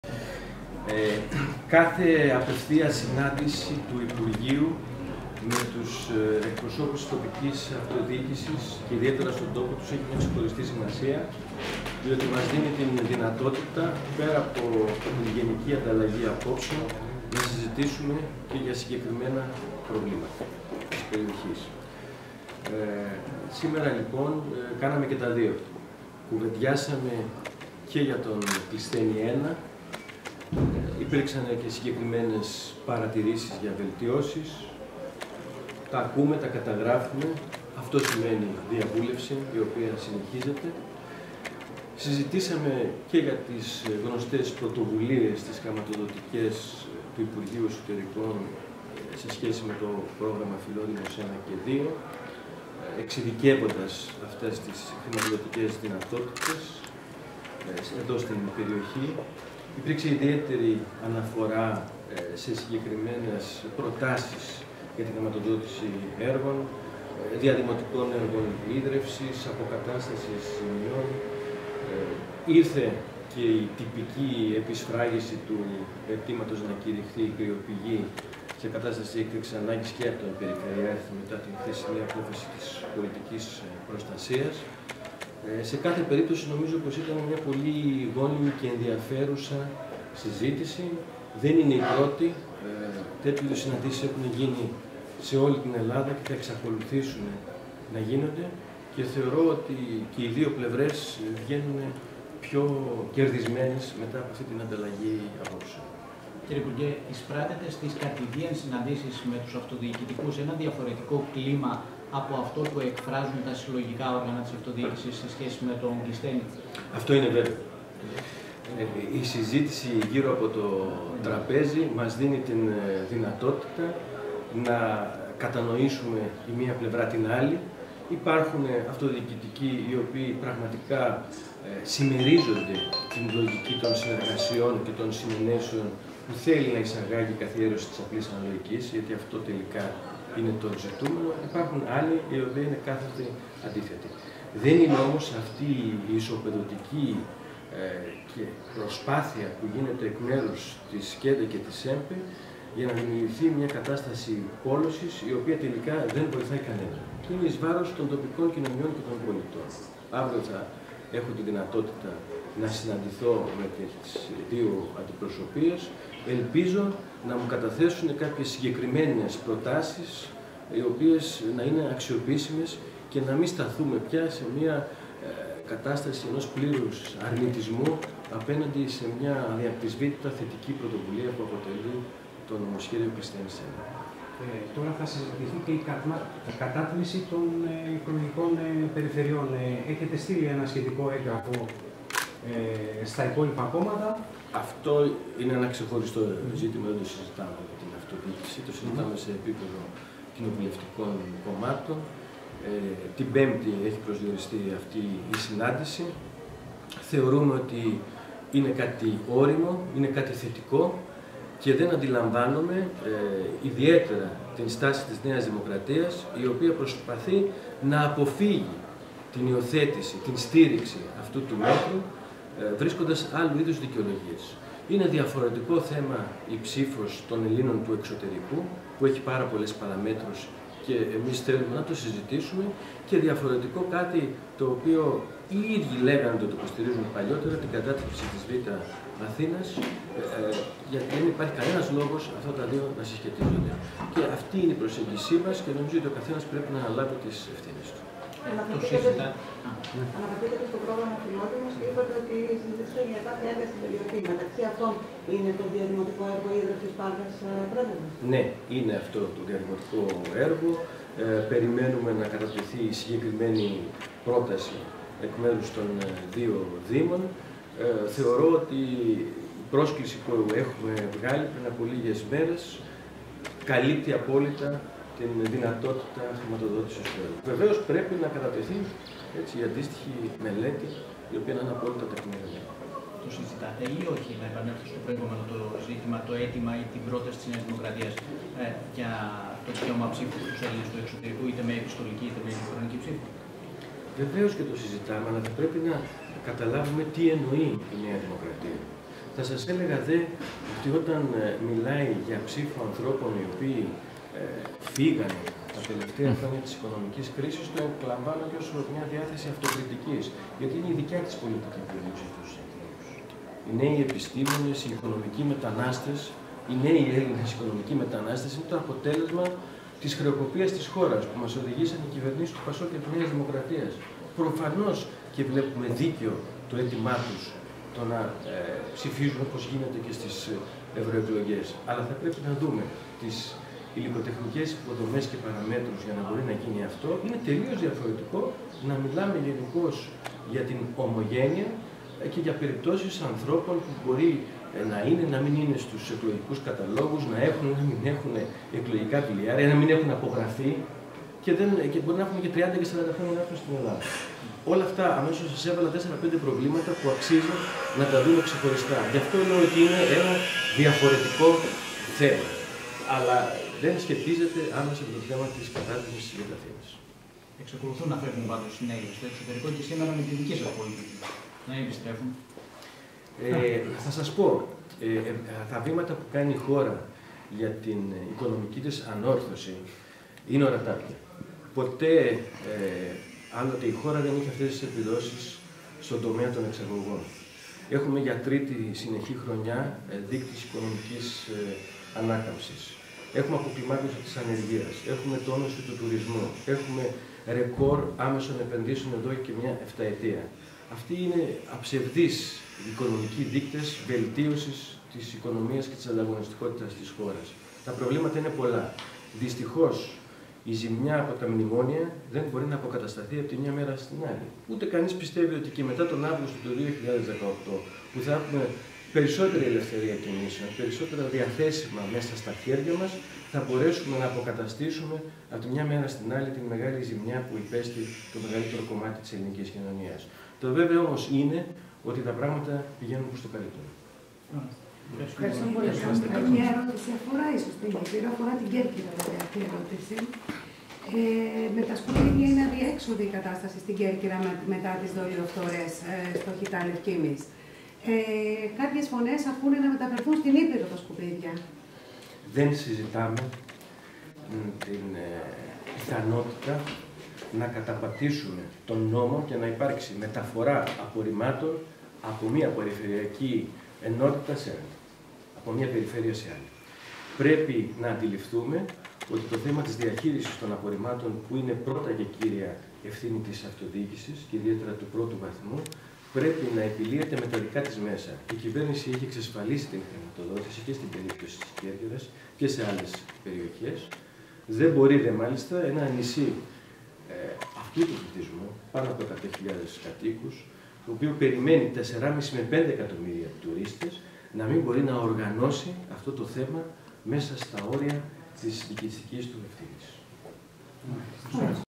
ΠΡΕΒΕΖΑ : Δηλώσεις του Υπουργού Εσωτερικών Πάνου Σκουρλέτη μετά την σύσκεψη με τους αιρετούς
MyPreveza, Η δική σου Πρέβεζα